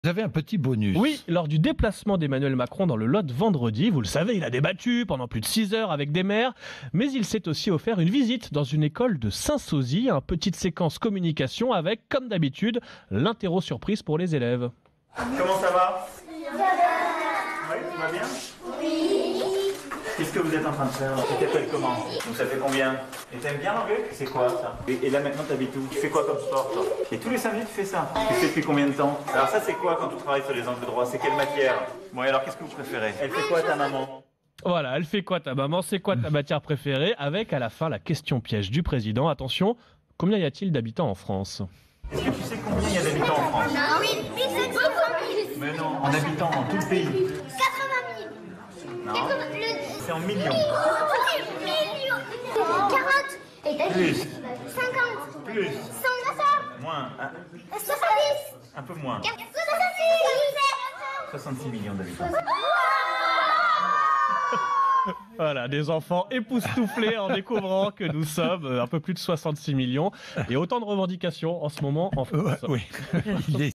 L'interro "surprise" d'Emmanuel Macron face à des petits écoliers
Lors du déplacement du Président dans le Lot à l'occasion du "grand débat", ce dernier a rencontré de jeunes élèves...